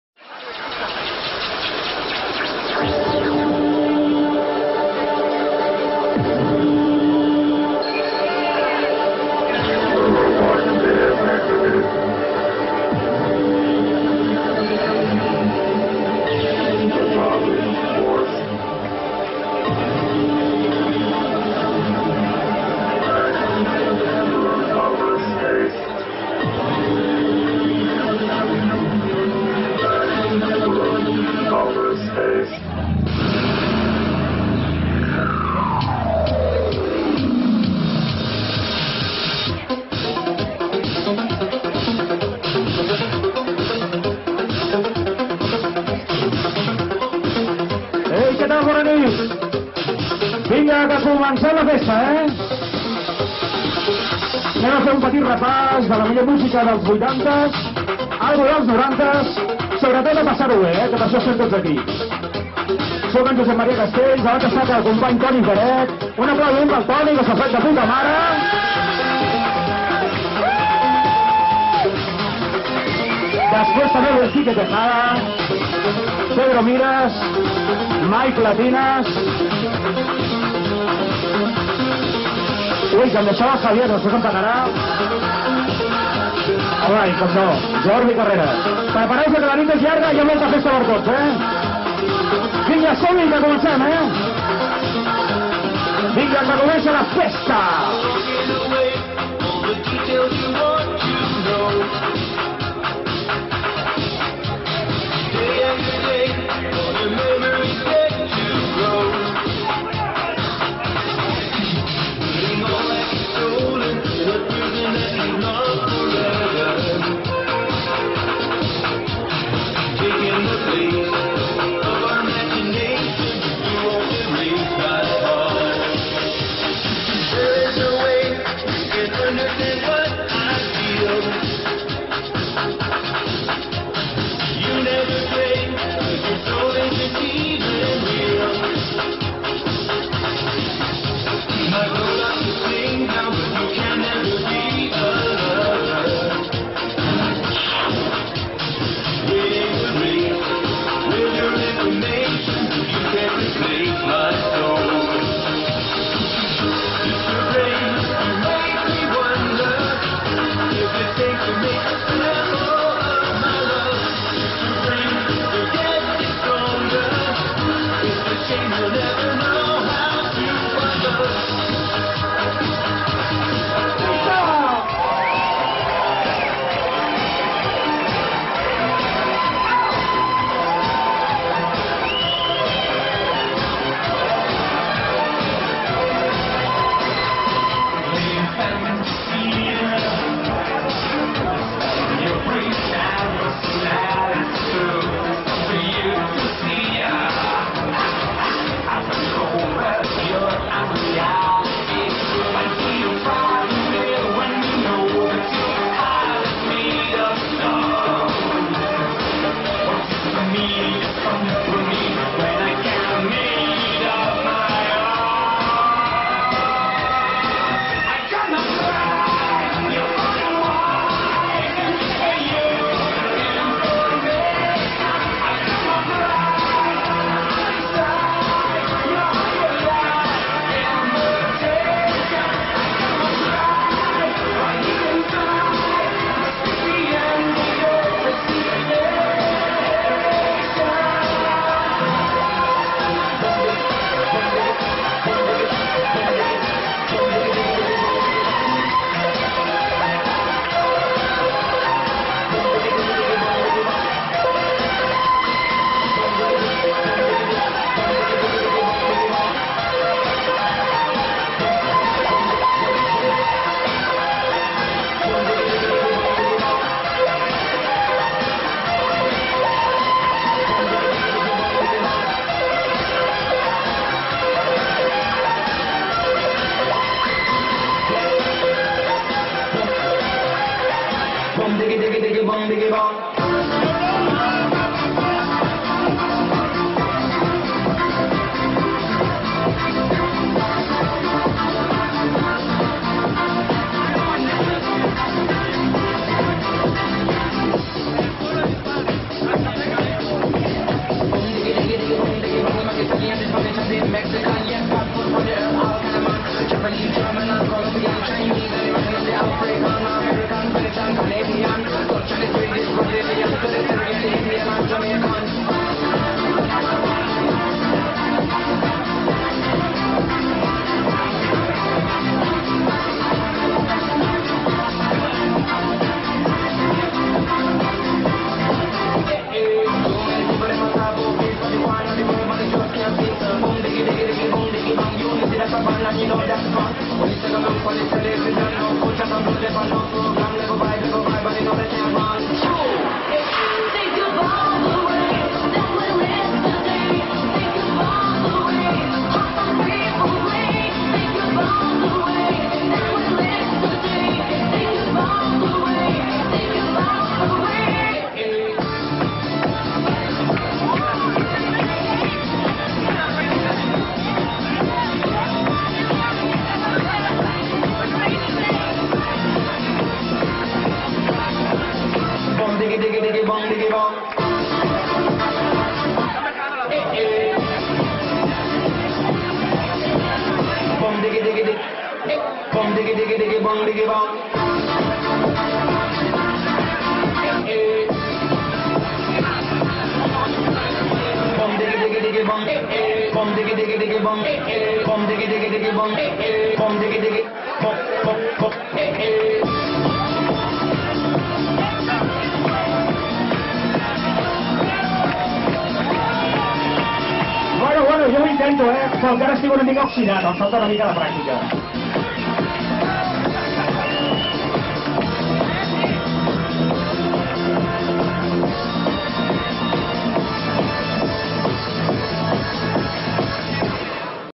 JOSE MARIA CASTELLS FIESTA MEGAMIX 2009 (GET BACK)
Pinchando en cabina de la discoteca GET BACK en la FIESTA MEGAMIX 2009.